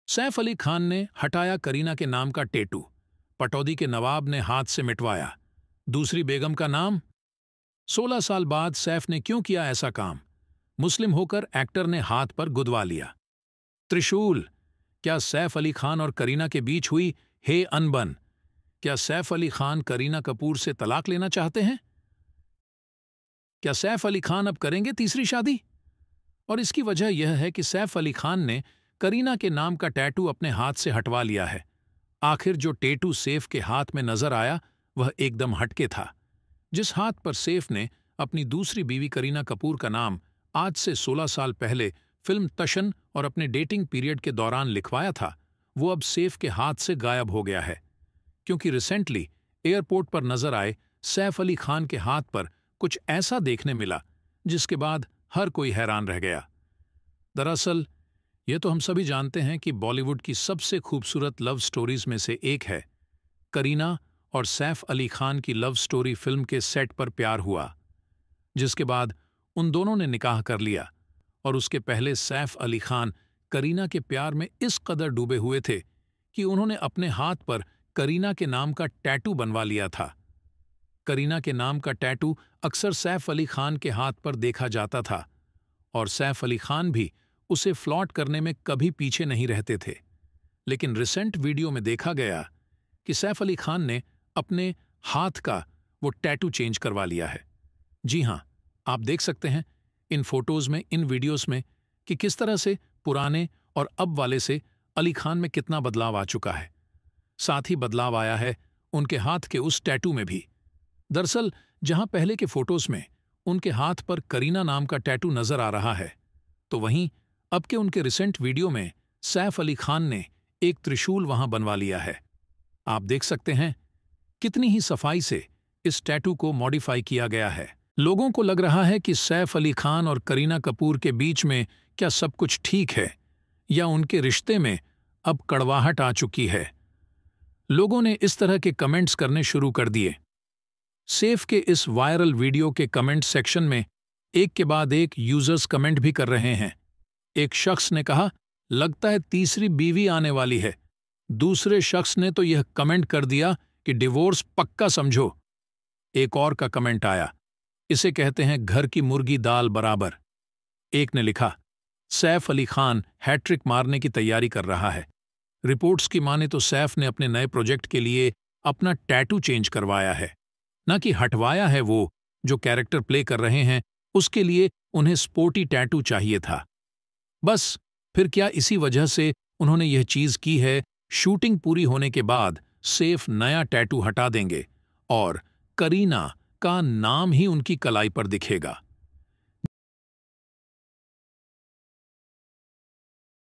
saif ने हटाया kareena के नामका टैटू |Fans बोले - तीसरी बेगम आयेगी ? | Saif Ali Khan Removd Tatoo
saif ने हटाया kareena के नामका टैटू |Fans बोले - तीसरी बेगम आयेगी ?| Saif Ali Khan Removd Tatoo Of Kareena Name | Saif Ali Khan | Kareena Kapoor Khan |